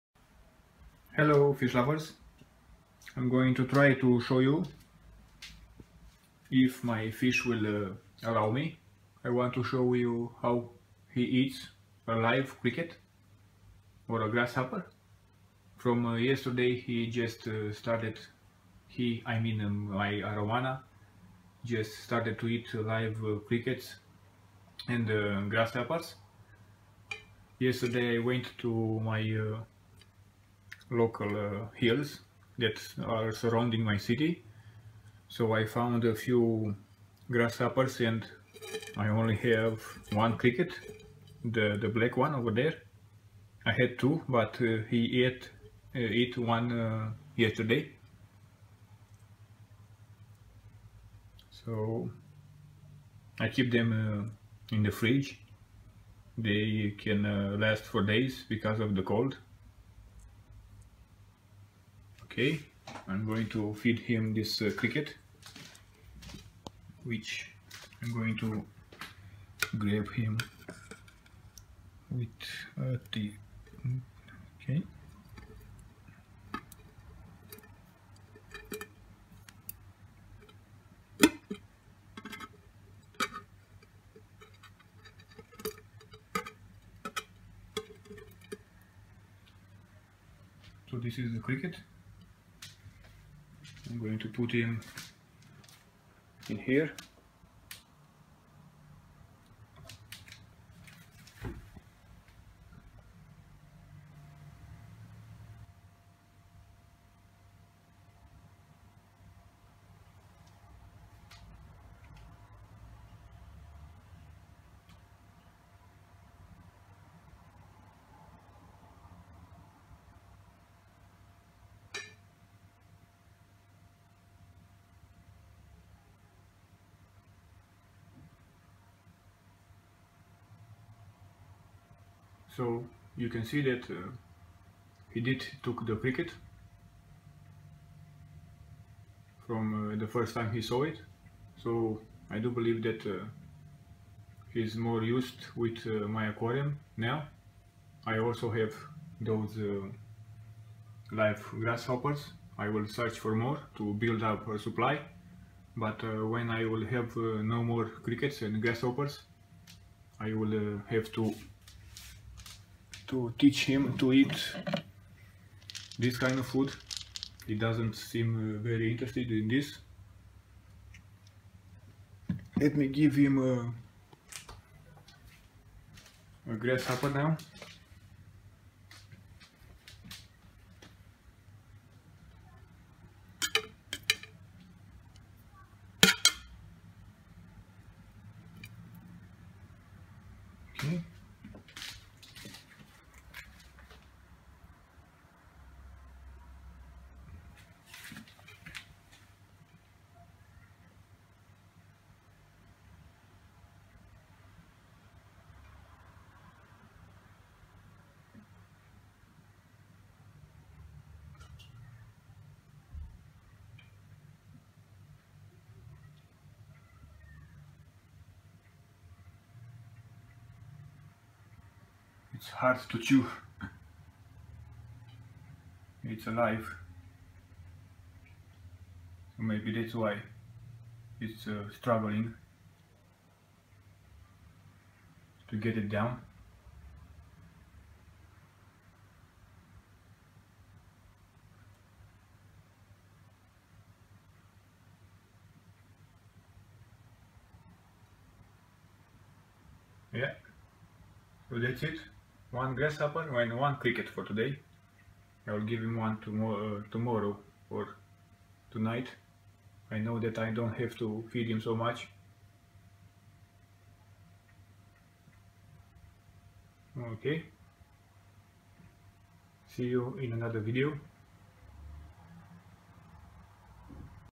Arowana eating crickets
Silver arowana eating live crickets and grasshoppers.